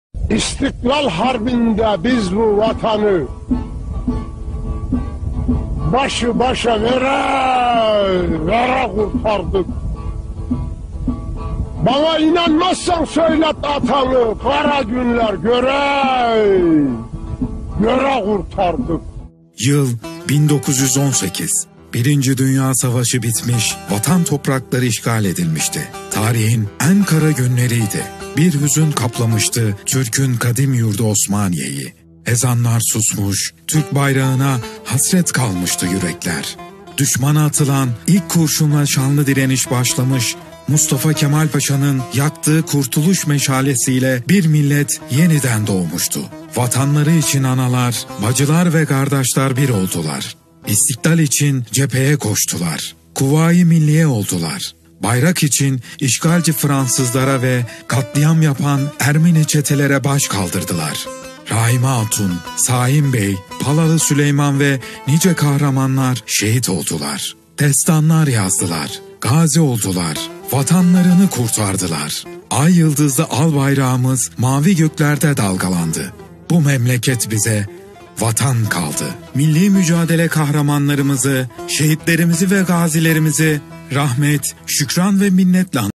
7 Ocak Osmaniye'nin Düşmandan Kurtuluşunun 99. Yılı
Osmaniye Valisi Dr. Erdinç Yılmaz: Başta Gazi Mustafa Kemal Atatürk'ü, milli mücadele kahramanlarımızı, bütün şehit ve gazilerimizi minnetle, şükranla yad ediyorum. Osmaniye'mizin kurtuluşu kutlu olsun.